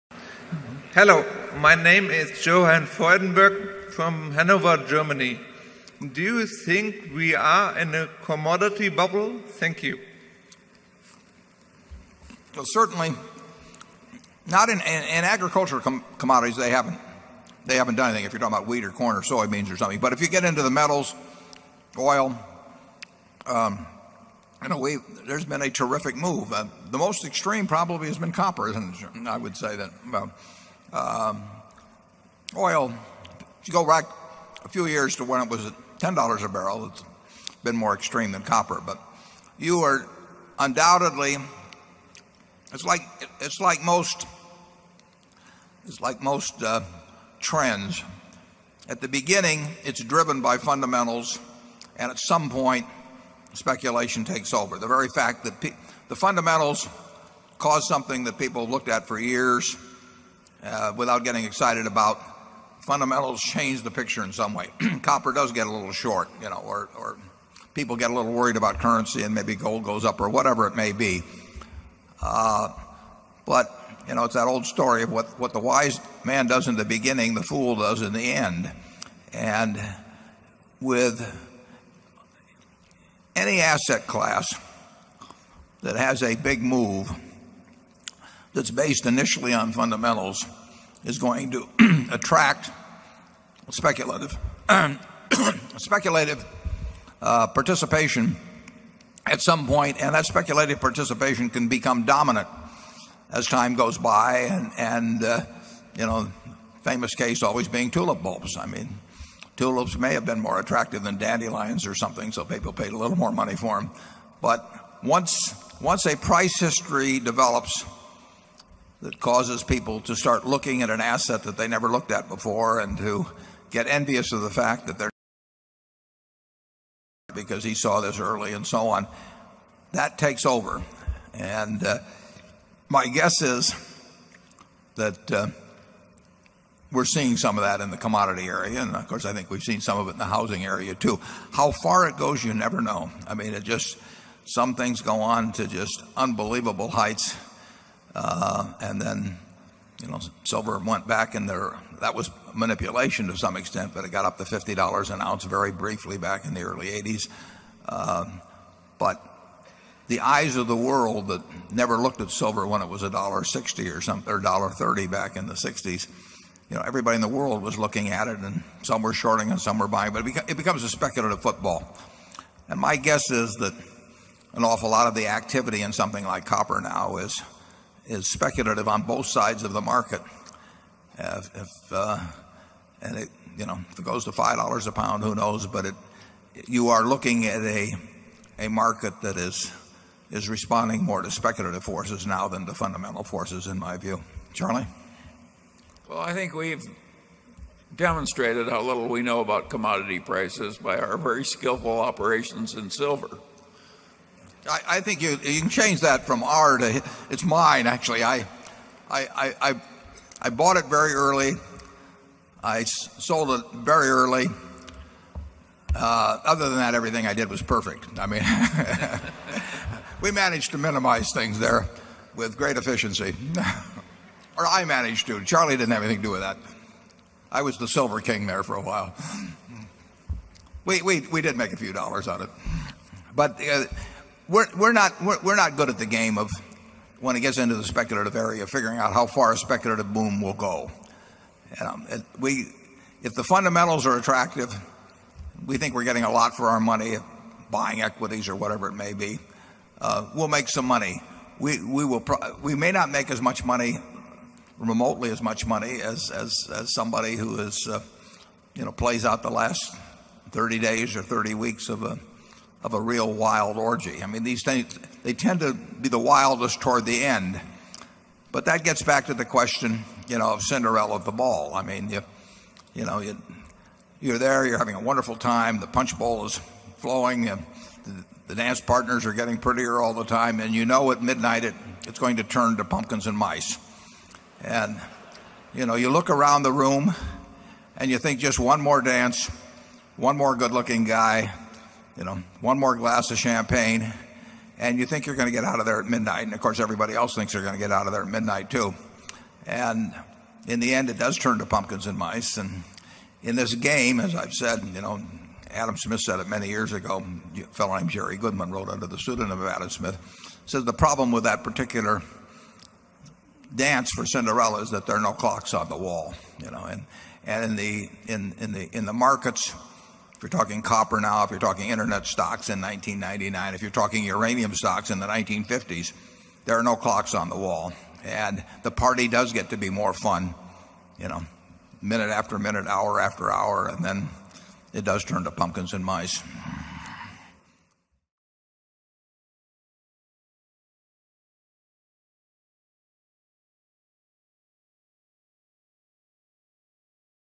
Spotting Bubble Trouble with Warren Buffett
(May 6, 2006) Bubbles continue to be obvious only in hindsight, largely because something based on a “sound premise” surely couldn’t be a bubble. Unfortunately in the markets, a premise is sound until it isn’t.

Warren Buffett was asked to comment on the bubble in the commodity market. As Mr. Buffett pointed out, "what the wise man does in the beginning, the fool does in the end." At the beginning of bubble, investors buy the commodity because of the strong fundamentals. Near the end of the bubble, speculators continue to buy the commodity because other speculators are buying the subject commodity.